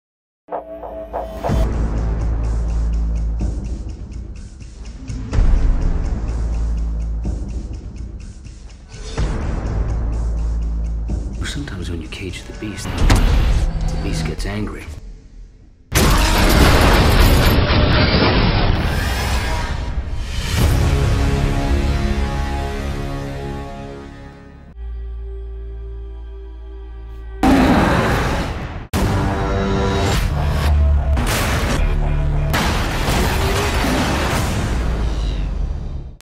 HULK VS WOLVERINE TEASER TRAILER (Live Action)
Fan made Live action trailer
My first attempt at masking